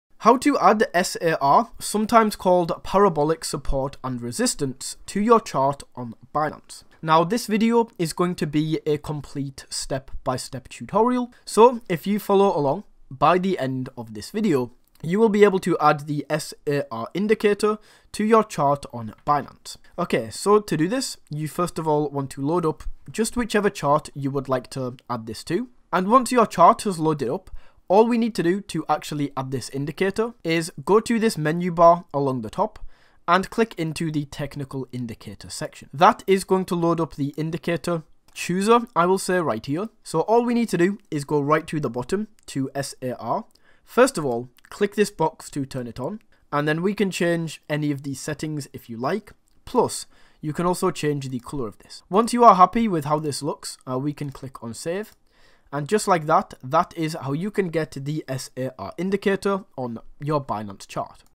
How To Add SAR Indicator to Chart on Binance
In this tutorial I will show you How To Add SAR Indicator to Chart on Binance